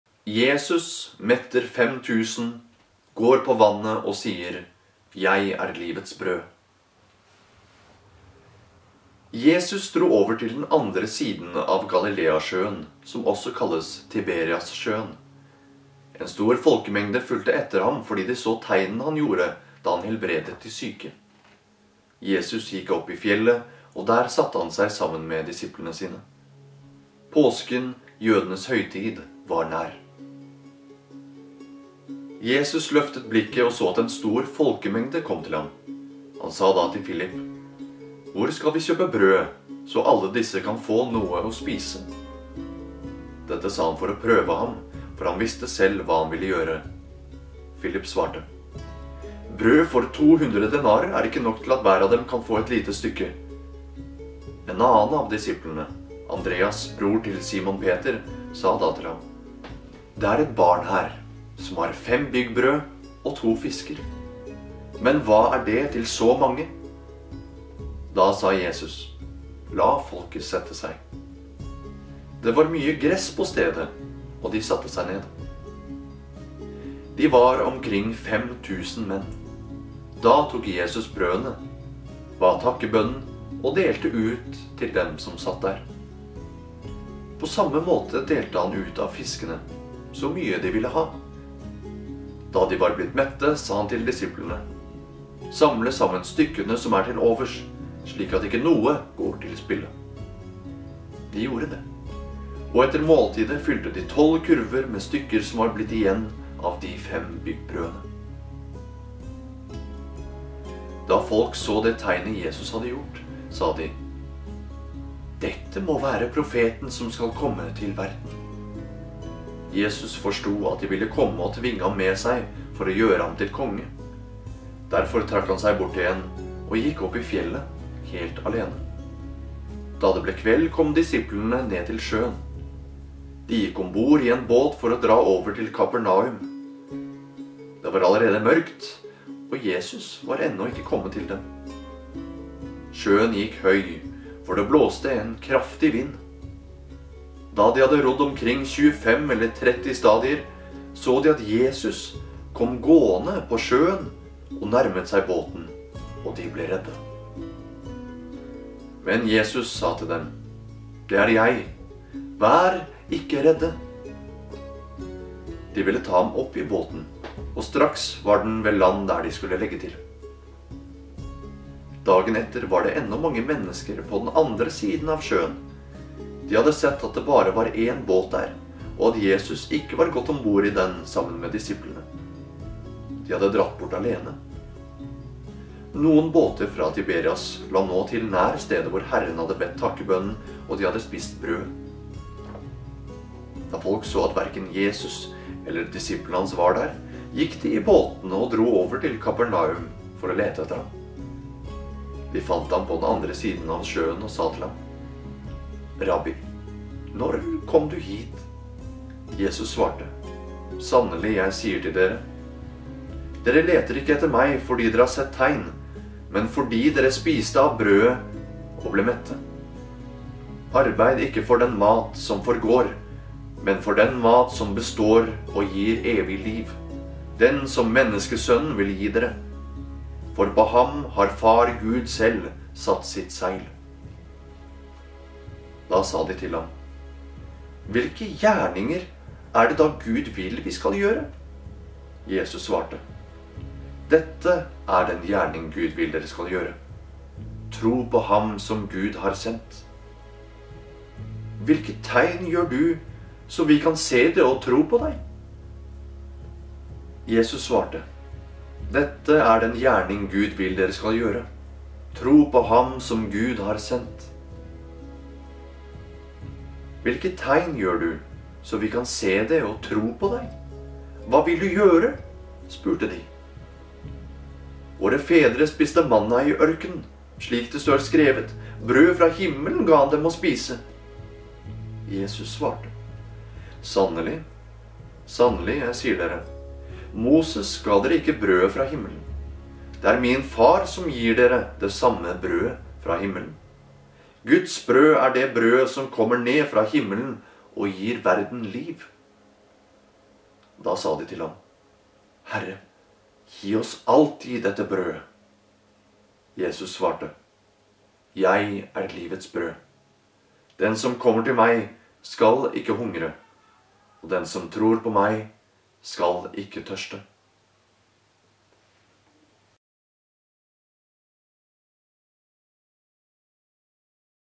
Jesus metter fem tusen, går på vannet og sier "Jeg er livet brød"
Henrik Bakken Flagestad